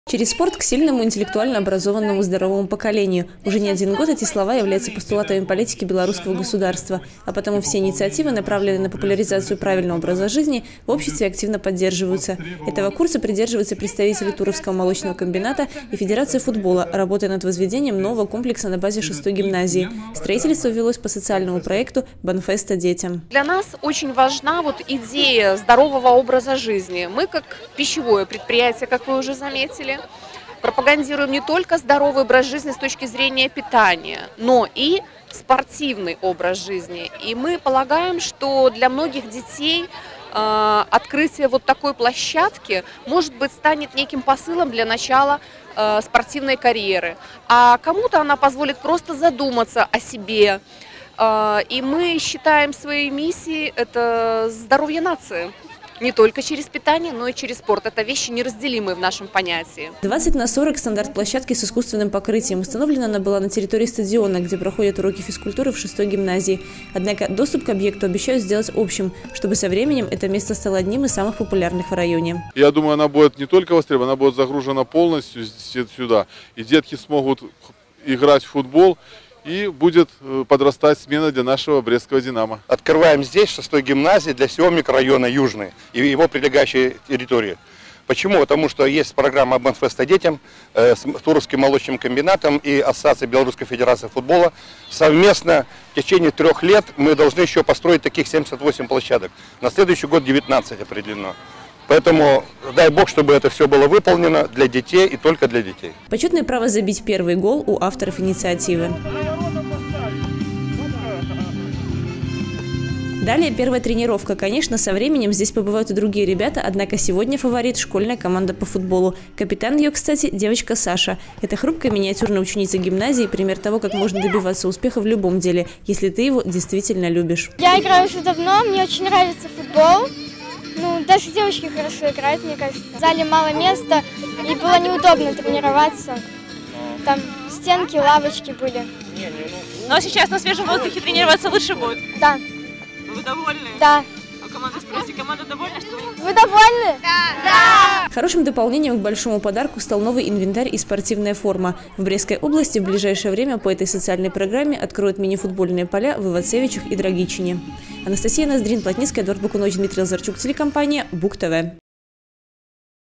2019-11-12 г. Брест. Открытие поля для мини-футбола. Новости на Буг-ТВ.
Открытие поля для мини-футбола в ГУО «Гимназия №6им. Г.К. Жукова».
Новый спортивный объект для юных брестчан появился в областном центре. При поддержке Туровского молочного комбината, Белорусской федерации футбола и местных органов власти на территории гимназии №6 была возведена площадка для мини-футбола. Теперь команда учреждения образования, в которую, к слову, входят и девочки, просто обязана побеждать: поле обустроена согласно всем требованиям времени, здесь созданы максимально удобные условия для занятия спортом